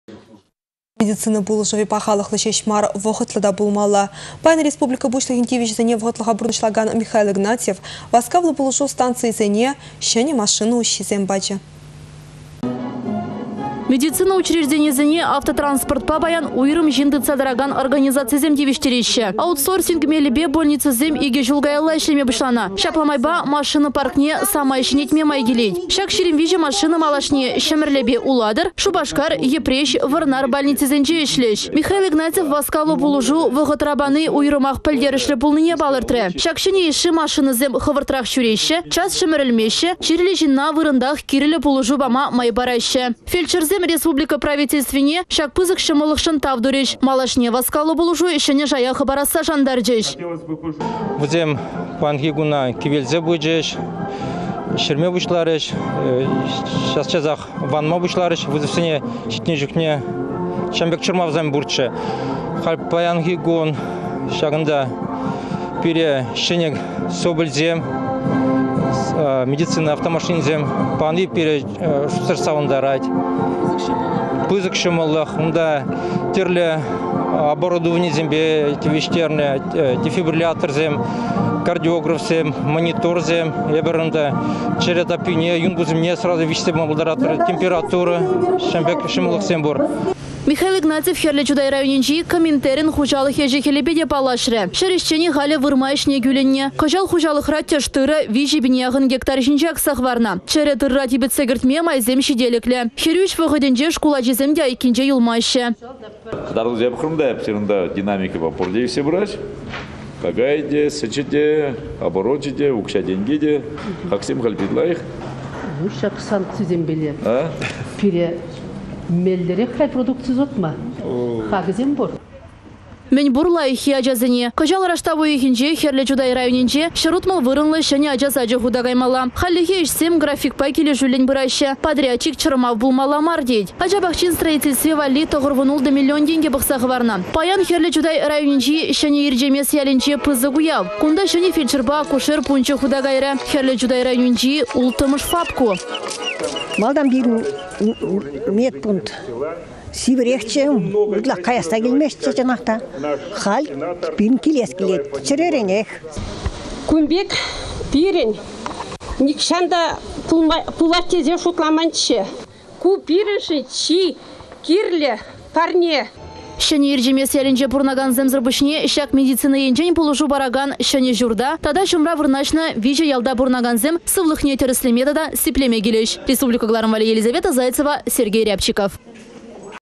Республикăра çĕнĕ социаллă пĕлтерĕшлĕ объектсем хута кайаççĕ
Медицина пулăшăвĕ пахалăхлă çеç мар, вăхăтлă та пулмалла. Паян Республика пуçлăхĕн тивĕçĕсене вăхăтлăха пурнăçлакан Михил Игнатьев васкавлă пулăшу станцийĕсене çĕнĕ машина уççисем пачĕ.